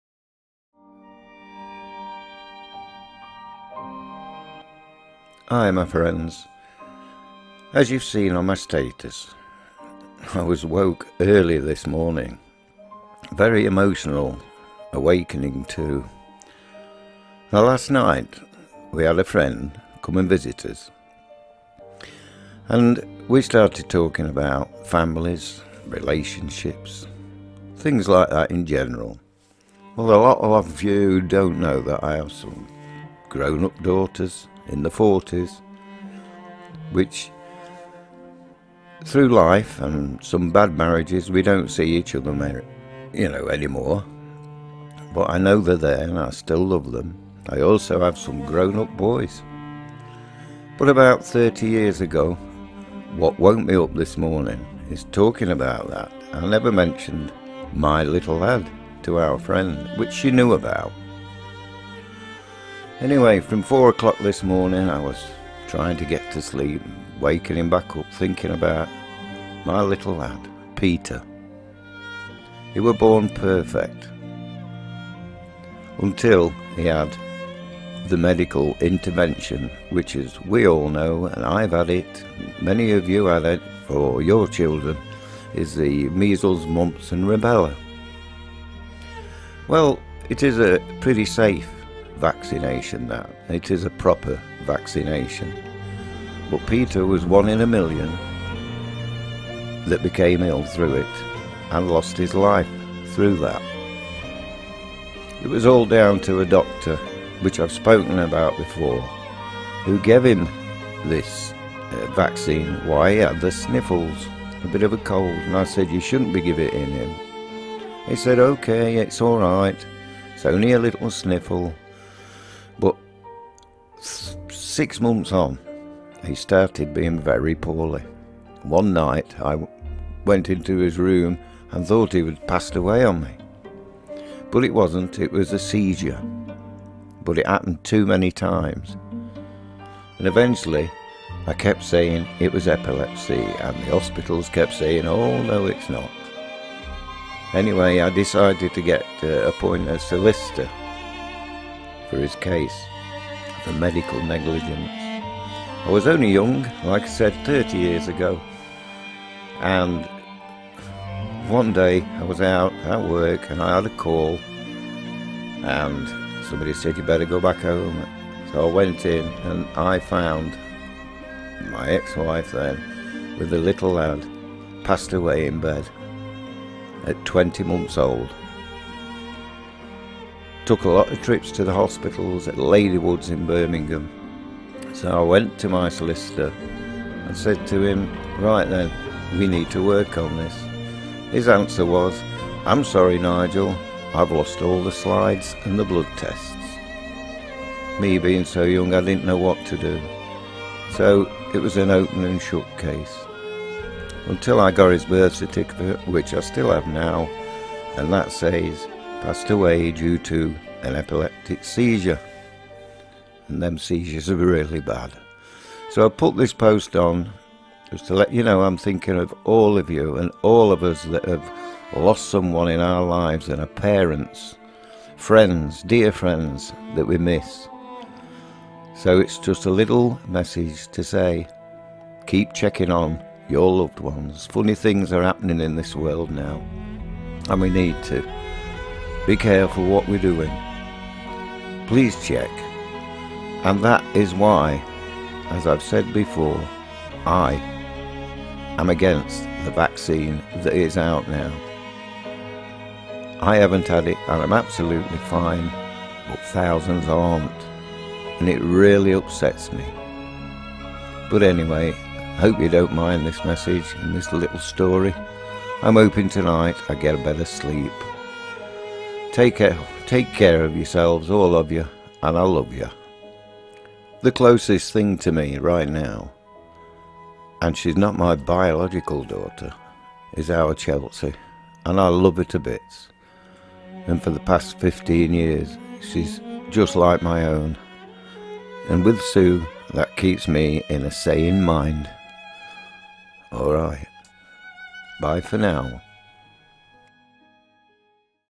THOUGHTS FROM LAST NIGHT.
My lost little boy Peter.
Had to put this little story up after a reckless night and a emotional wakening at silly O'clock. Not looking for sympathy but just a post to anyone that's been through the same thing. Check on your loved ones.
Thinking of you all..